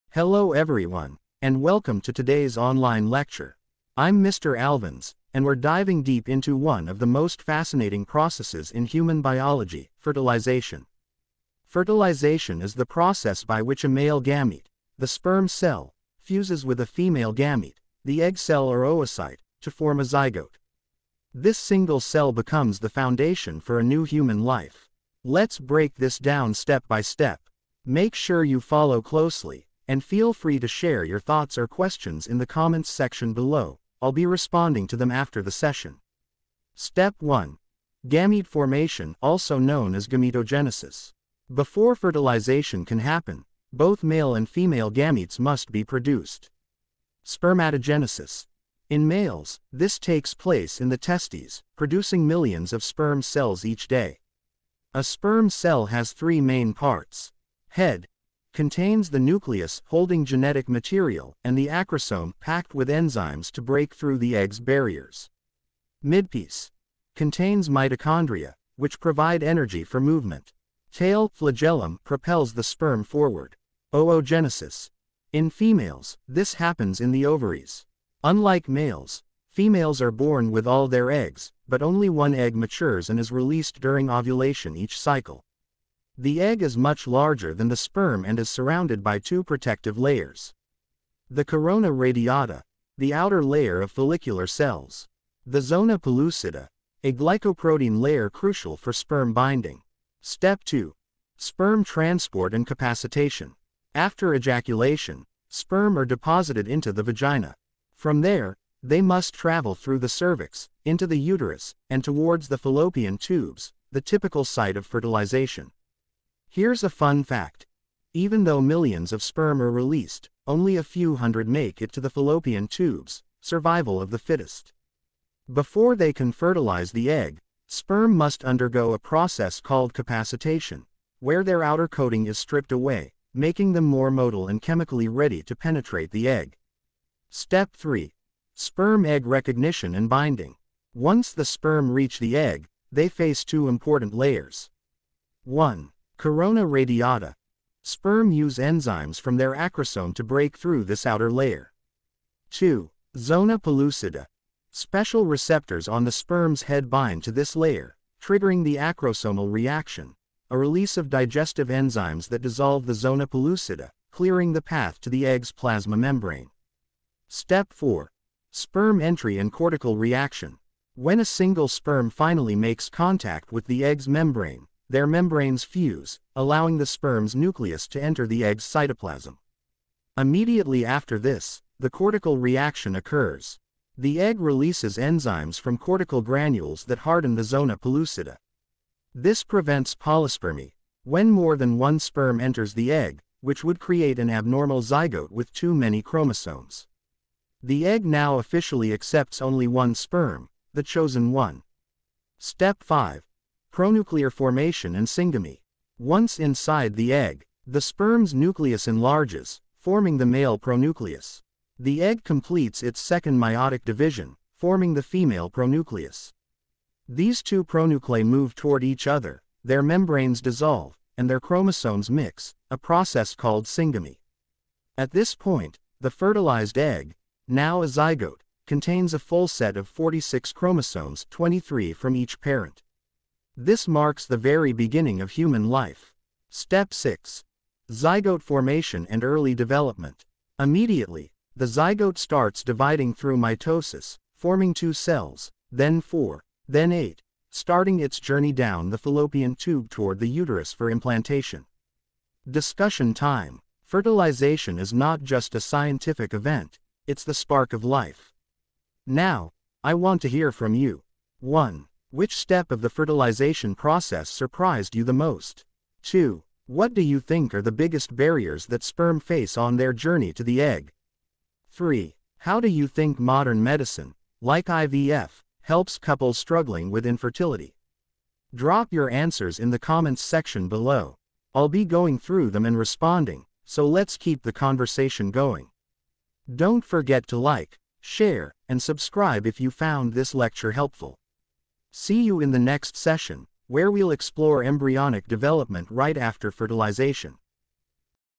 Fertilization Process Explained | How Sperm Meets Egg
0:00 Introduction to Fertilization
1:15 Sperm Journey
2:45 Egg Fertilization
4:10 Zygote Formation